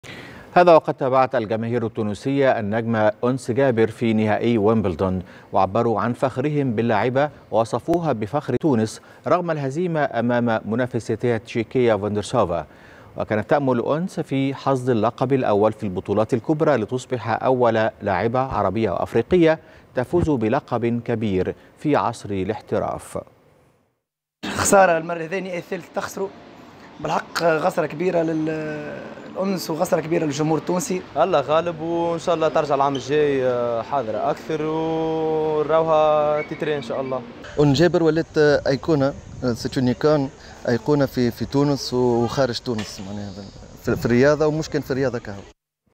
جماهير تونس تعبر عن فخرها رغم خسارة أنس جابر لقب ويمبلدون
15/7/2023